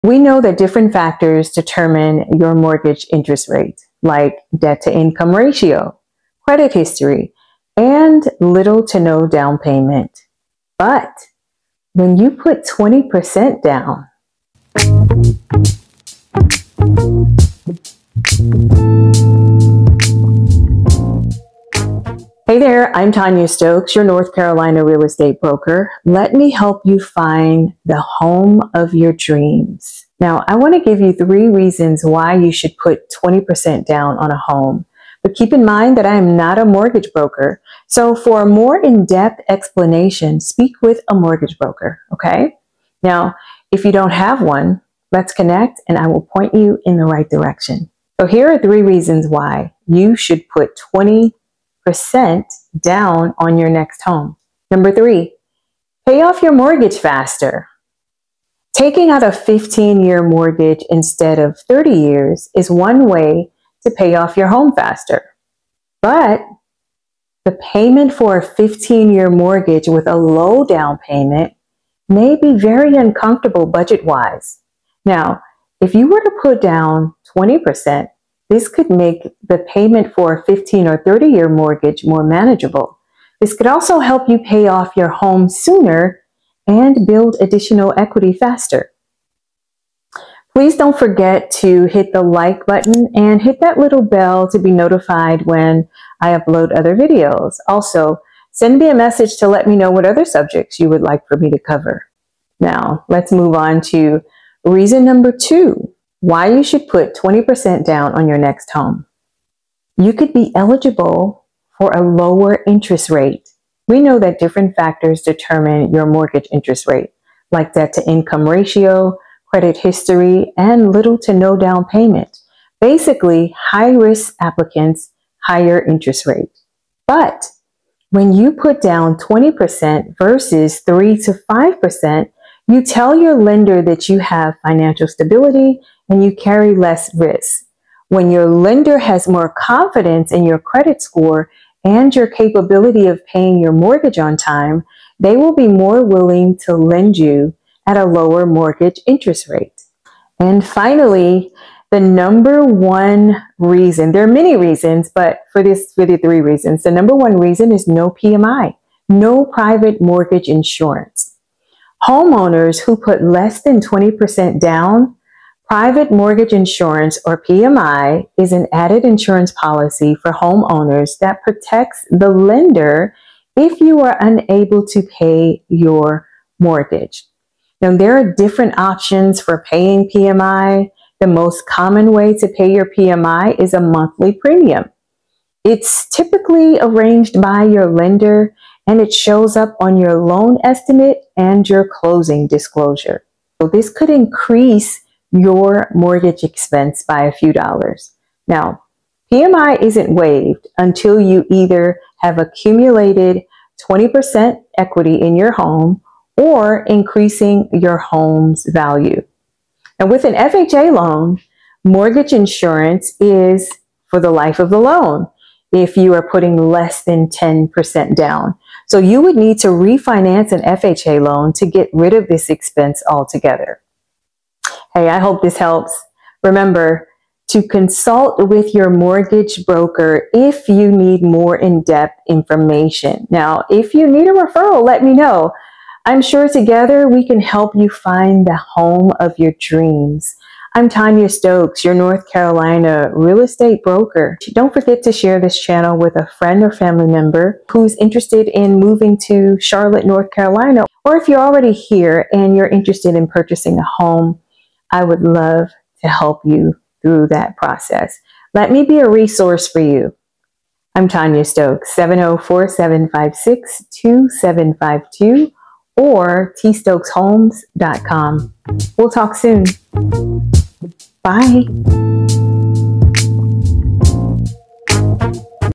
Three Reasons Why You Should Put 20% Down on Your Home.
Although  there are many loan products  available that don't require  20% down. I want to share  a few reasons  you may want to consider it.